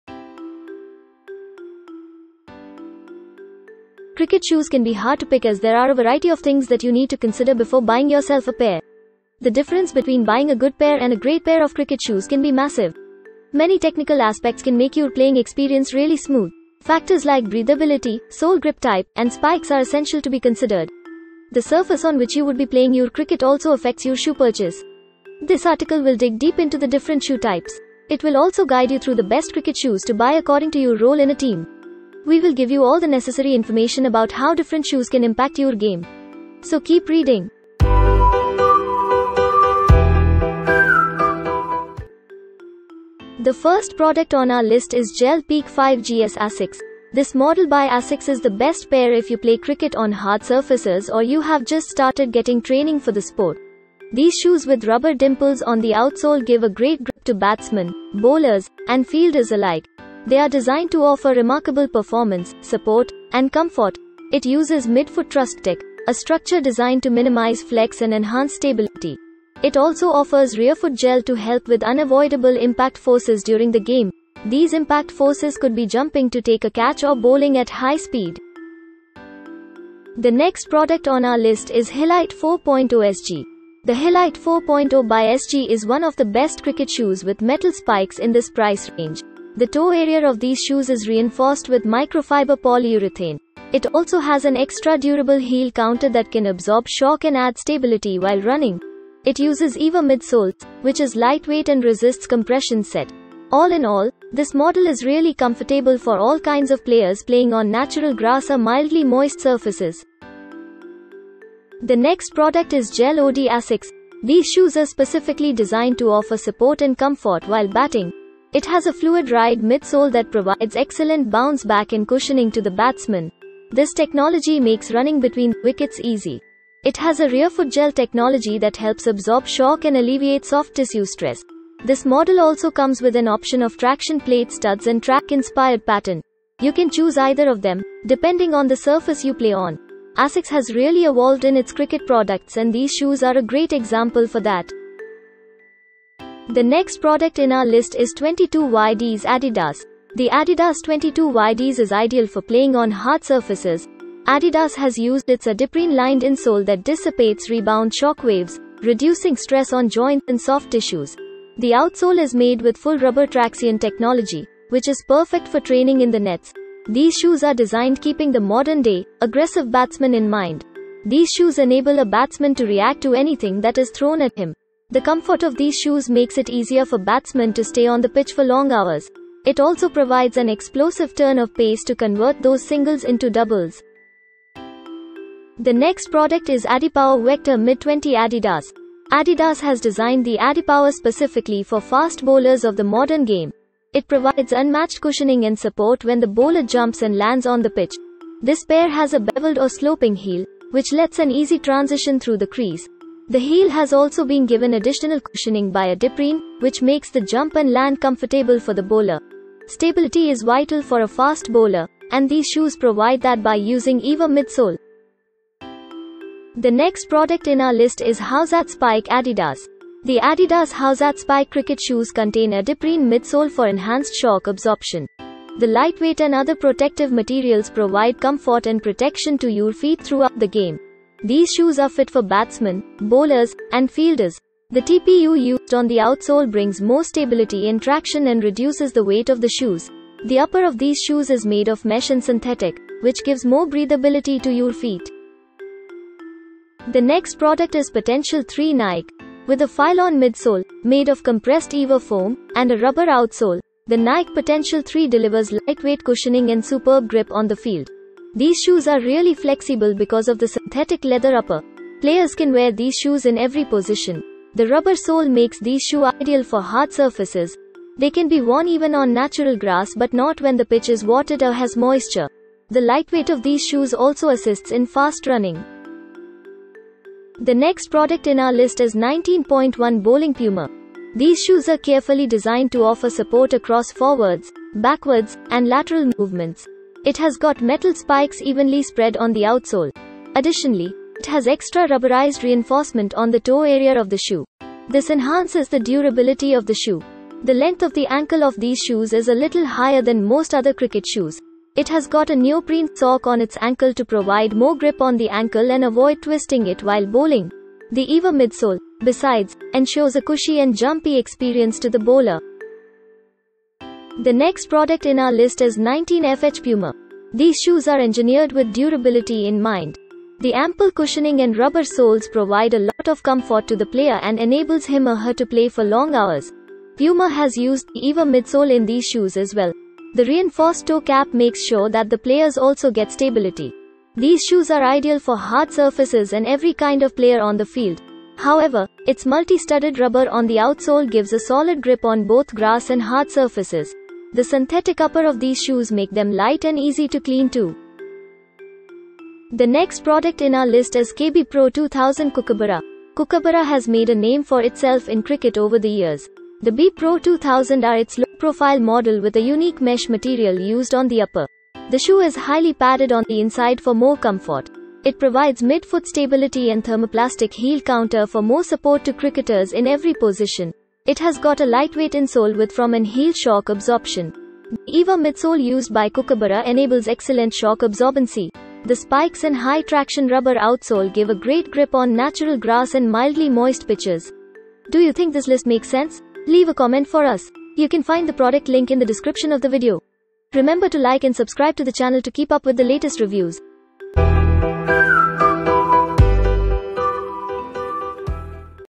🌵 10 Best Cricket Shoes in India (ASICS, Adidas, and more)
🌵 10 Best Cricket Shoes in India (ASICS, Adidas, and more) | Lemongrass. Here are our editor picks:

00:00 Introduction

00:41 #1 - GEL-PEAKE 5 GS ASICS (From ₹4,999.00)

01:18 #2 - Hilite 4.0 SG (From ₹4,028.00)

01:49 #3 - GEL-ODI ASICS (From ₹5,599.00)

02:24 #4 - 22 Yds Adidas (From ₹9,940.00)

03:03 #5 - Adipower Vector Mid 20 Adidas (From ₹11,999.00)

03:34 #6 - Howzat Spike Adidas (From ₹6,450.00)

04:04 #7 - Potential 3 Nike (From ₹4,490.00)

04:35 #8 - 19.1 Bowling Puma (From ₹6,999.00)

05:12 #9 - 19 Fh Puma (From ₹5,499.00)

05:47 #10 - KB Pro 2000 Kookaburra (From ₹3,268.00)

06:24 Ending

Cricket shoes can be hard to pick as there are a variety of things that you need to consider before buying yourself a pair. The difference between buying a good pair and a great pair of cricket shoes can be massive. Many technical aspects can make your playing experience really smooth. Factors like breathability, sole grip type, and spikes are essential to be considered.
The surface on which you would be playing your cricket also affects your shoe purchase. This video will dig deep into the different shoe types. It will also guide you through the best cricket shoes to buy according to your role in a team. We will give you all the necessary information about how different shoes can impact your game. So keep reading!